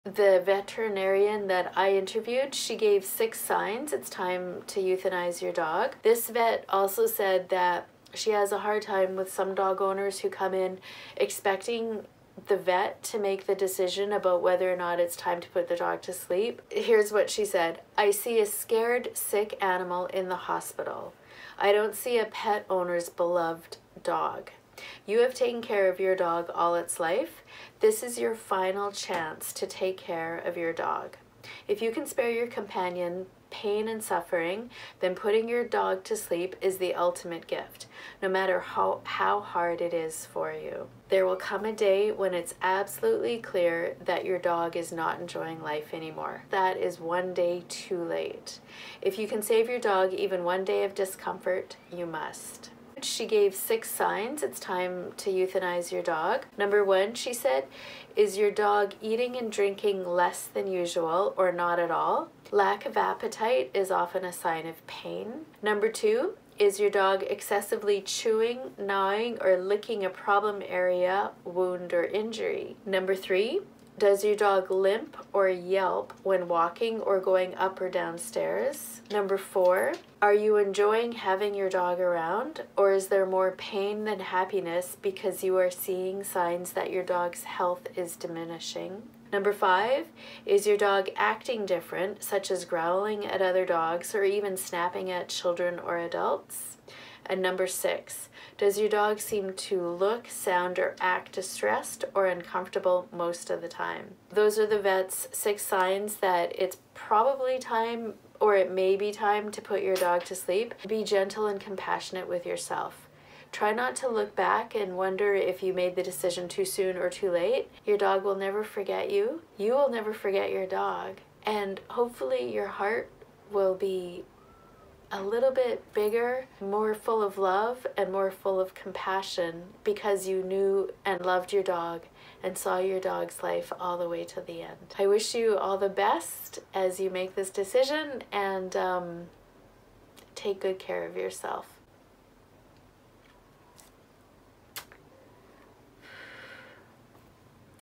Should You Put Your Dog to Sleep? 6 Signs an Animal is Suffering
Dogs - all animals - hide their pain. It's a survival instinct that makes it hard for us to know if we should put our dogs to sleep. But, as veterinarian Dr Marie Haynes says, "If you can save your dog even one day of discomfort, you must."  These signs it's time to put your dog to sleep are based on her professional and personal experience with sick, elderly dogs. Euthanizing a dog you love is a very difficult decision, but here's the most important thing to remember: putting your dog to sleep is the final act of love and compassion you give your dog.

Deciding if you should put your dog down because of an illness or injury is one of the hardest decisions you'll ever make.

That's the number one sign it’s time to put your dog to sleep: if suffering, pain, or a low quality of life is involved.  Answering the question “what is a low quality of life for a dog?” is different for everyone…but the bottom line is that if your dog is suffering in any way, then it’s time to say goodbye.

Deciding if it's time to put your dog to sleep is sometimes a clear decision. Other times, we need a veterinarian's advice and insight about the right time to say good-bye. Sometimes a dog is in so much pain, so injured, or so old that the only and best option is to put him to sleep. Most of the time, though, the decision to euthanize a dog – who is a treasured, loved part of the family – is not easy.

There are no easy signs, and the definition of “good quality of life for a dog” is different for every pet owner. Plus, as one veterinarian told me, it’s often difficult to tell whether a dog is in pain or suffering. But, there are a few signs it really is time to put your dog to sleep.

This veterinarian gave six signs it is time to put a dog to sleep:

1. Is your dog eating and drinking less than usual, or not at all? Lack of appetite is often a sign of pain.
2. Is your dog excessively chewing, gnawing, or licking a problem area, wound, or injury?
3. Does your dog limp or yelp when walking or going up/downstairs?
4. Are you enjoying having your dog around…or is there more pain than happiness because you are seeing signs your dog`s health is diminishing ?
5. Is your dog acting different, such as growling at other dogs or even snapping at children or adults?
6. Does your dog seem to look, sound, or act distressed or uncomfortable most of the time?

These questions are not always easy to answer. Pet owners often want veterinarians or other dog experts to tell them if it is time to put their dog to sleep. Pet owners do not want the responsibility of putting their dogs to sleep too soon…or even too late.

It can be tempting to ask the veterinarian or other dog experts to decide if it’s time, but our vet said this has to be the pet owner’s decision. The vet only sees a snapshot of the dog’s life, while the pet owner has the big perspective and the whole lifespan.

“I see a scared, sick animal in the hospital,” says Dr Haynes. “I don’t see a pet owner’s beloved dog. You have taken care of your dog all its life. This is your final chance to take care of your dog.  If you can spare your companion pain and suffering, then putting your to sleep is the ultimate gift…no matter how hard it is for you.”

How will you know if it is time to put your dog to sleep? Here’s what the veterinarian says: There will come a day when it is absolutely clear to you that your dog is not enjoying life. That day is one day too late. If you can save your dog even one day of discomfort, you must.

Deciding on euthanasia is difficult, but it could be the most loving thing you do for your dog.

With love and sympathy,
Laurie